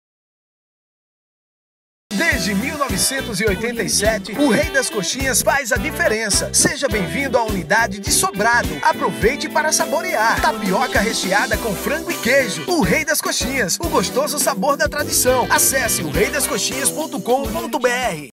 VT Comercial O Rei das Coxinhas Sobrado - PB
Bom dia!! Penúltimo comercial da série O REI DAS COXINHAS. O oitavo é de Sobrado na Paraíba. Compartilhe a tradição do sabor! Assista agora! @oreidascoxinhas @mplaybrasil @marcello.mancha @pedrosanttosps